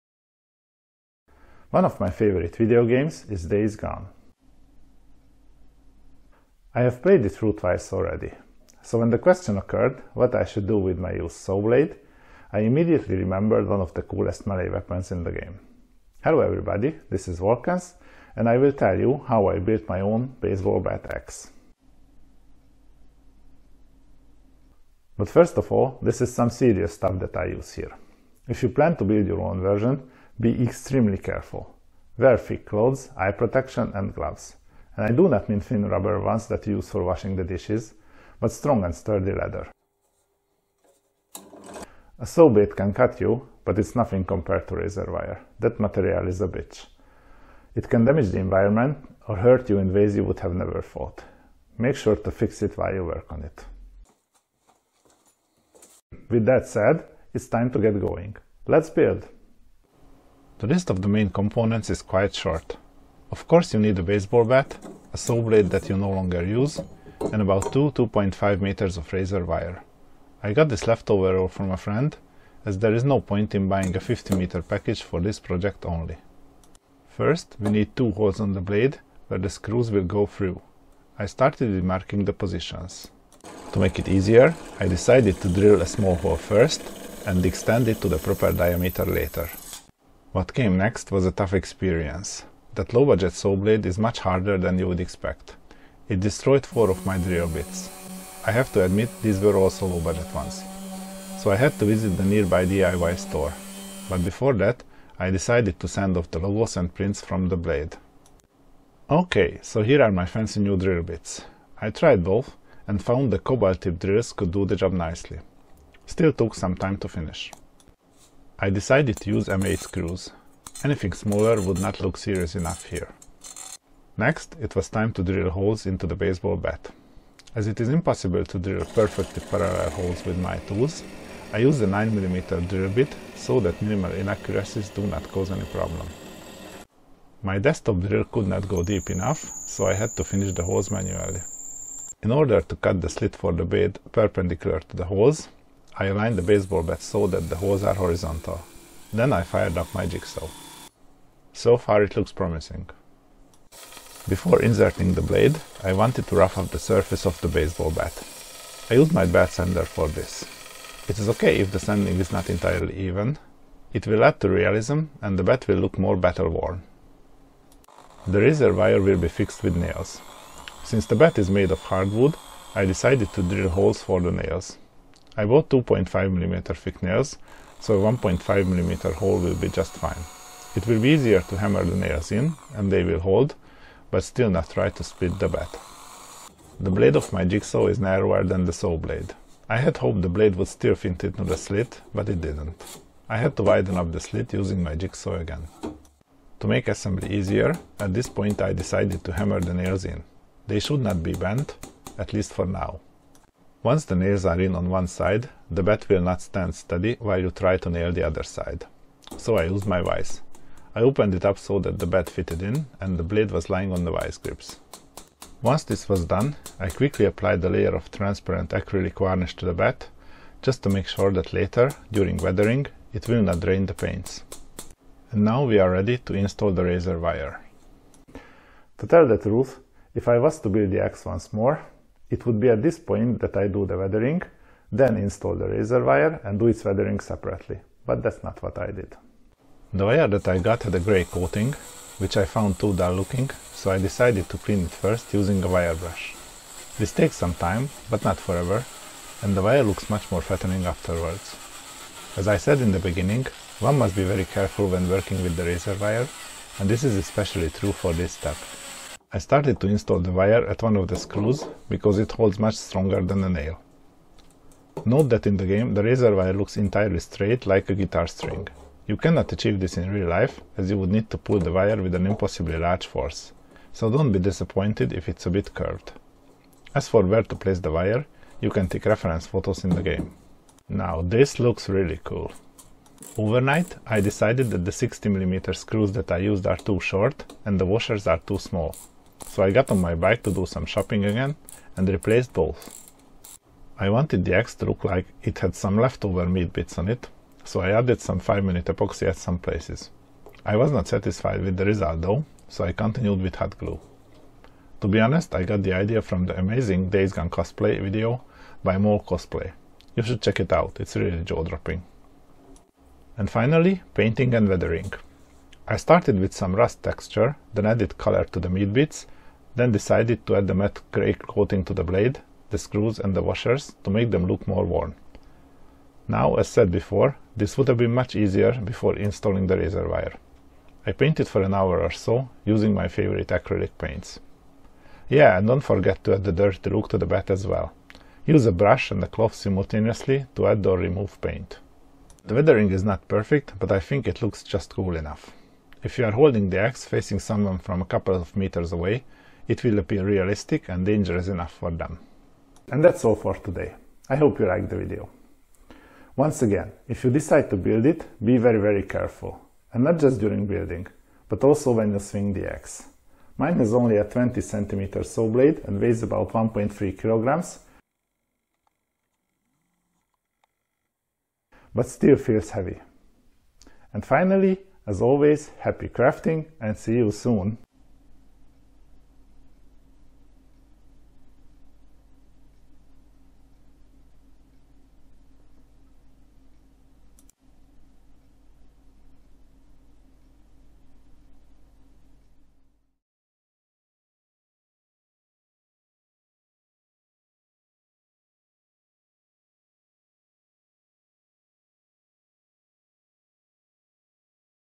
How to make the baseball bat axe from Days gone
In this video I'll show you how to make the baseball bat axe from the video game Days gone.